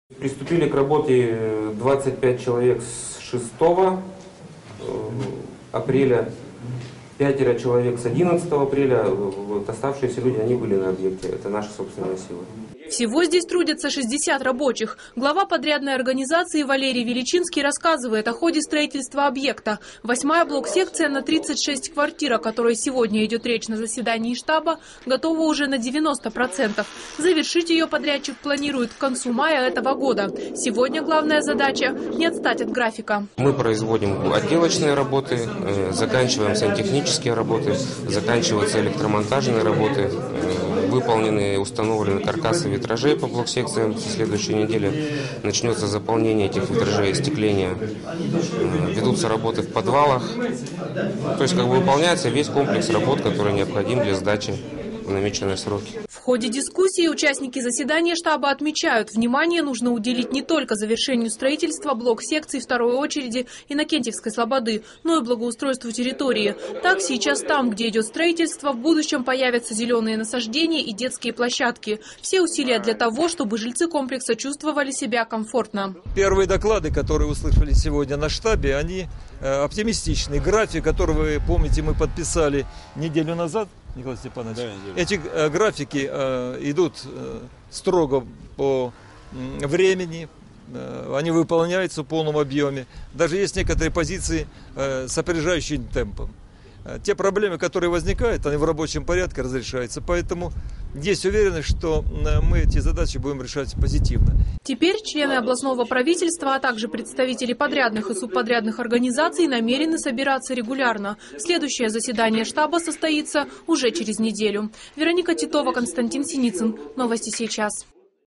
Иннокентьевская слобода.flv
Строительство второй очереди Иннокентьевская слободы идет по графику. Ход работ на этом объекте теперь будет контролировать специально созданный штаб, в который вошли члены областного правительства и представители подрядных и субподрядных организаций. Штаб создан по распоряжению Губернатора Дмитрия Мезенцева. Сегодня состоялось выездное заседание - на стройплощадке, где сейчас возводят очередную блок-секцию.